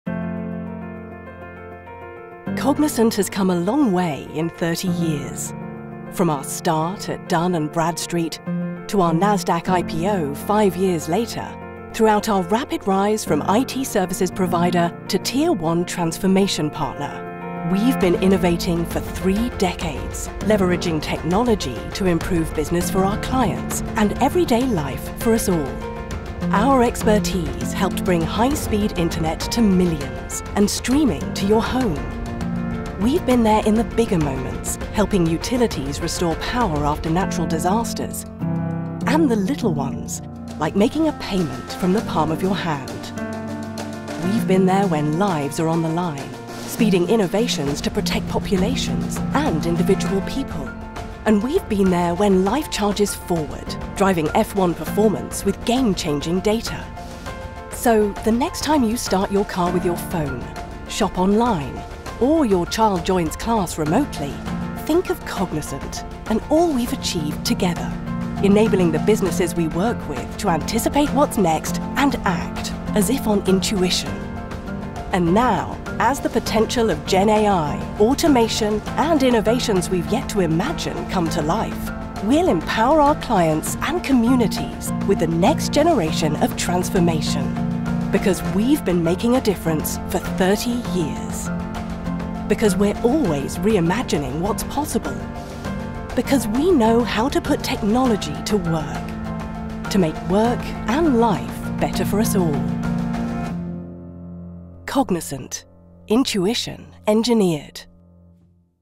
Celebrating 30 years | Cognizant
Learn about Cognizant’s 30 years of innovation at the confluence of technology and industry, improving business for our clients and everyday life for us all.